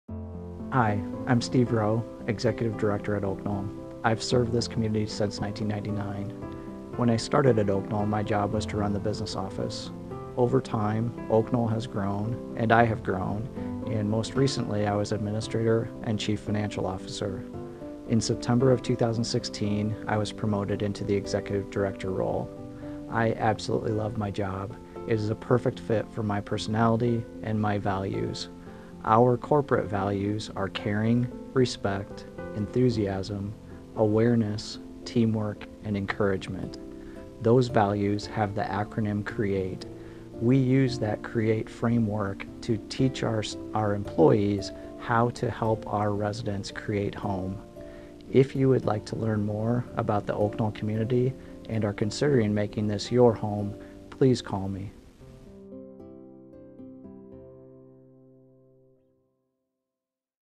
Steve Roe
Oaknoll Executive Director, Steve Roe, talks about his ascension to the  Executive Director position and Oaknoll's CREATE values.